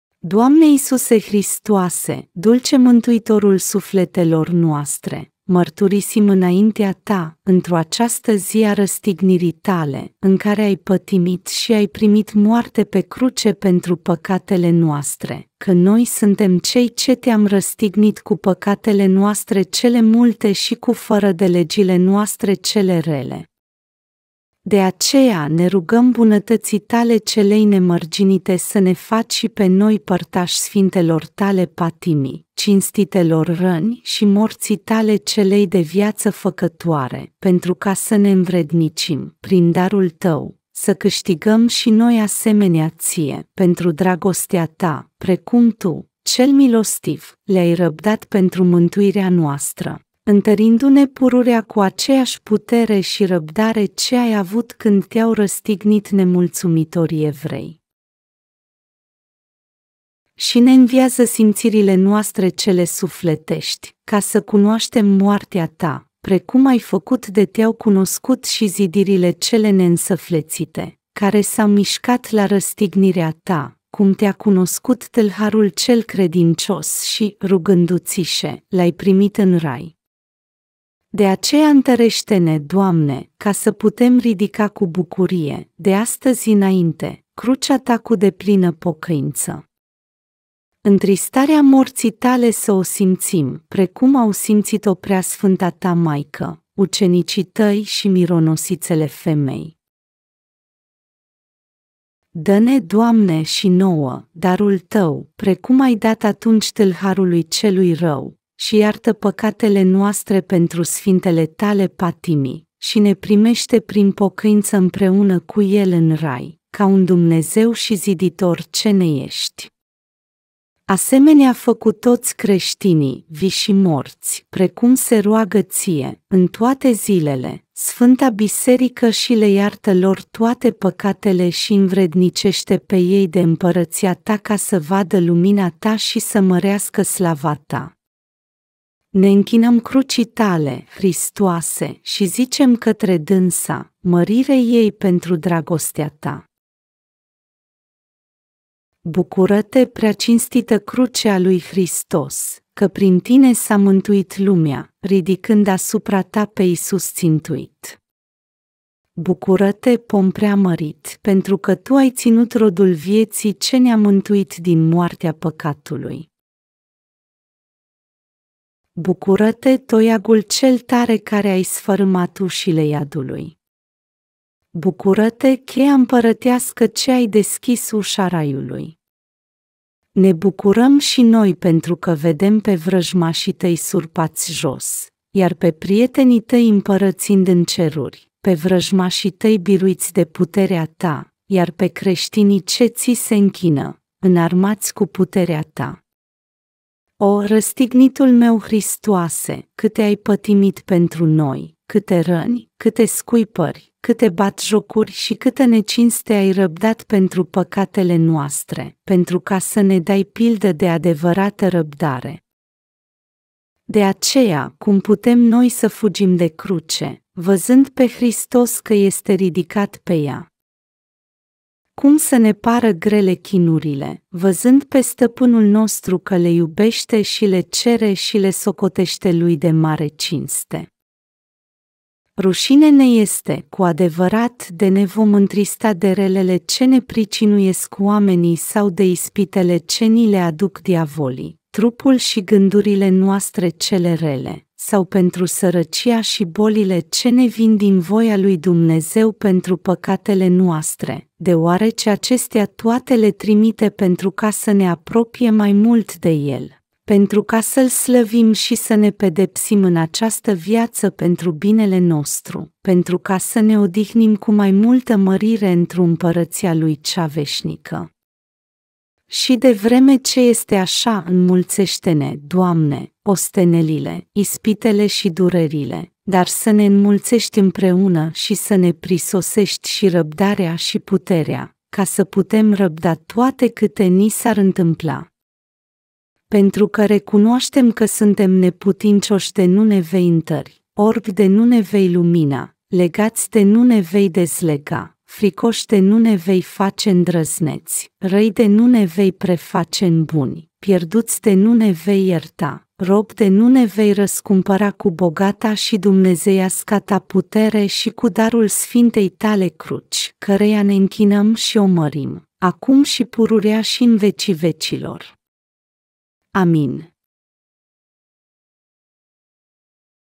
Rugaciune SPECIALA pentru iertarea pacatelor catre Domnul Nostru Iisus Hristos
Doamne Iisuse Hristoase, dulce Mântuitorul sufletelor noastre, mărturisim înaintea Ta, întru această zi a răstignirii Tale, în care ai pătimit și ai primit moarte pe Cruce pentru păcatele noastre, că noi suntem cei ce Te-am răstignit cu păcatele noastre cele multe și cu fărădelegile noastre cele rele. De aceea, ne rugăm bunătății Tale celei nemărginite să ne faci și pe noi părtași sfintelor Tale Patimi, cinstitelor răni și morții Tale celei de viață făcătoare, pentru ca să ne învrednicim, prin darul Tău, să câștigăm și noi asemenea Ție, pentru dragostea Ta, precum Tu, Cel Milostiv, le-ai răbdat pentru mântuirea noastră, întărindu-ne pururea cu aceeași putere și răbdare ce ai avut când Te-au răstignit nemulțumitorii evrei.

Și ne înviază simțirile noastre cele sufletești, ca să cunoaștem moartea Ta, precum ai făcut de Te-au cunoscut și zidirile cele neînsuflețite, care s-au mișcat la răstignirea Ta, cum Te-a cunoscut tâlharul cel credincios și, rugându-Ți-se, l-ai primit în Rai. De aceea întărește-ne, Doamne, ca să putem ridica cu bucurie, de astăzi înainte, Crucea Ta cu deplină pocăință. Întristarea morții Tale să o simțim, precum au simțit-o Preasfânta Ta Maică, ucenicii Tăi și mironosițele femei.

Dă-ne, Doamne, și nouă, darul Tău, precum ai dat atunci tâlharului celui rău, și iartă păcatele noastre pentru sfintele Tale Patimi, și ne primește prin pocăință împreună cu el în Rai, ca un Dumnezeu și Ziditor ce ne ești. Asemenea fă cu toți creștinii, vii și morți, precum se roagă Ție, în toate zilele, Sfânta Biserică și le iartă lor toate păcatele și-i învrednicește pe ei de Împărăția Ta ca să vadă lumina Ta și să mărească slava Ta. Ne închinăm Crucii Tale, Hristoase, și zicem către dânsa: mărire ei pentru dragostea Ta.

Bucură-te, Preacinstită Cruce a lui Hristos, că prin tine s-a mântuit lumea, ridicând asupra ta pe Iisus țintuit. Bucură-te, pom preamărit, pentru că Tu ai ținut Rodul vieții Ce ne-a mântuit din moartea păcatului.

Bucură-te, toiagul cel tare care ai sfărâmat ușile iadului. Bucură-te, cheia împărătească ce ai deschis ușa Raiului. Ne bucurăm și noi pentru că vedem pe vrăjmașii Tăi surpați jos, iar pe prietenii Tăi împărățind în ceruri, pe vrăjmașii Tăi biruiți de puterea Ta, iar pe creștinii ce Ți se închină, înarmați cu puterea Ta. O, Răstignitul meu Hristoase, câte ai pătimit pentru noi, câte răni, câte scuipări, câte batjocuri și câtă necinste ai răbdat pentru păcatele noastre, pentru ca să ne dai pildă de adevărată răbdare! De aceea, cum putem noi să fugim de Cruce, văzând pe Hristos că este ridicat pe ea? Cum să ne pară grele chinurile, văzând pe Stăpânul nostru că le iubește și le cere și le socotește Lui de mare cinste? Rușine ne este, cu adevărat, de ne vom întrista de relele ce ne pricinuiesc oamenii sau de ispitele ce ni le aduc diavolii, trupul și gândurile noastre cele rele, sau pentru sărăcia și bolile ce ne vin din voia lui Dumnezeu pentru păcatele noastre, deoarece acestea toate le trimite pentru ca să ne apropie mai mult de El, pentru ca să-L slăvim și să ne pedepsim în această viață pentru binele nostru, pentru ca să ne odihnim cu mai multă mărire întru Împărăția Lui cea veșnică. Și, de vreme ce este așa, înmulțește-ne, Doamne, ostenelile, ispitele și durerile, dar să ne înmulțești împreună și să ne prisosești și răbdarea și puterea, ca să putem răbda toate câte ni s-ar întâmpla. Pentru că recunoaștem că suntem neputincioși de nu ne vei întări, orbi de nu ne vei lumina, legați de nu ne vei dezlega, fricoși de nu ne vei face îndrăzneți, răi de nu ne vei preface în buni, pierduți de nu ne vei ierta, robi de nu ne vei răscumpăra cu bogata și dumnezeiasca Ta putere și cu darul Sfintei Tale Cruci, căreia ne închinăm și o mărim, acum și pururea și în vecii vecilor. Amin.